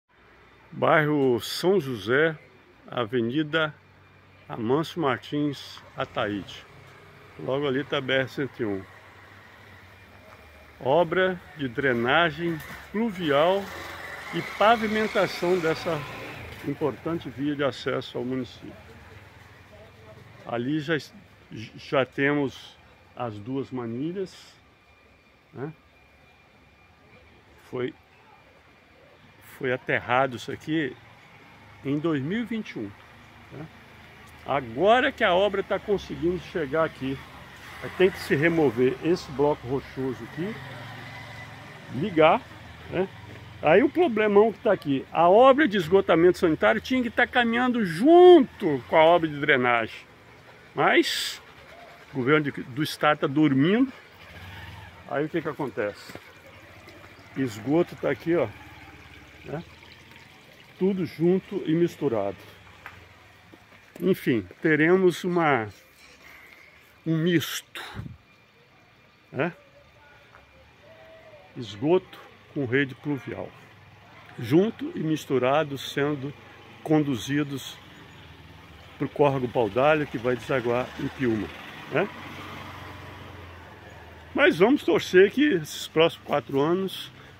ESGOTO E REDE PLUVIAL JUNTOS E MISTURADOS EM RIO NOVO DO SUL-ES, ATÉ QUANDO PREFEITO E GOVERNADOR??
*RIO NOVO DO SUL-ES* .
 _Até quando rede de drenagem pluvial e de esgoto caminharão juntos e misturados??_
 *Com a palavra o Prefeito Nei CASTELARI e o Governador RENATO CASAGRANDE.*
Helio Scheidegger
Vereador-PL